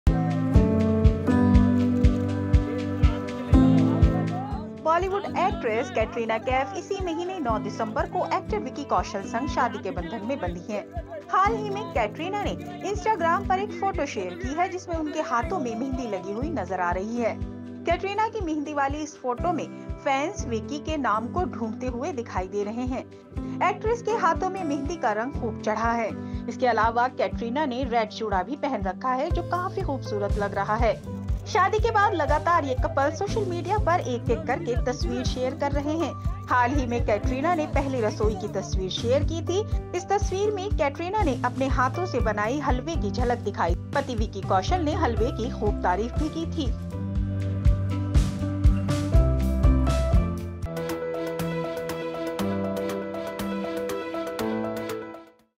Katrina Kaif ने दिखाई अपनी खूबसूरत मेहंदी, Vicky Kaushal का नाम ढूंढते हुए नजर आए फैंस
बॉलीवुड एक्ट्रेस कैटरीना कैफ (Katrina Kaif) इसी महीने 9 दिसंबर को एक्टर विक्की कौशल (Vicky Kaushal) के साथ शादी के बंधन में बंधी हैं. हाल ही में कैटरीना ने इंस्टाग्राम पर एक फोटो शेयर की है जिसमें उनके हाथों में मेहंदी लगी हुई नजर आ रही है.

जैसे ही कैटरीना ने इस फोटो को शेयर किया,फोटो जमकर वायरल होने लगी. कैटरीना की मेहंदी वाली इस फोटो में फैंस विक्की (Vicky) के नाम को ढूंढते हुए दिखाई दे रहे हैं.

एक्ट्रेस के हाथों में मेहंदी का रंग खूब चढ़ा है. इसके अलावा कैटरीना ने रेड चूड़ा भी पहन रखा है जो काफी खूबसूरत लग रहा है. कैटरीना ने जो तस्वीर शेयर की है उसके बैकग्राउंड में समुद्र दिखाई दे रहा है. इस फोटो को देख फैंस यही कयास लगा रहे हैं कि ये तस्वीर विक्की और कैटरीना के हनीमून की है.

शादी के बाद लगातार ये कपल सोशल मीडिया पर एक-एक कर तस्वीरें शेयर कर रहा है. हाल ही कैटरीना कैफ ने पहली रसोई की तस्वीर शेयर की थी.

इस तस्वीर में कैट ने अपने हाथों से बनाई हलवे की झलक दिखाई थी. पति विक्की कौशल (Vicky Kaushal) ने हलवे की खूब तारीफ भी की थी.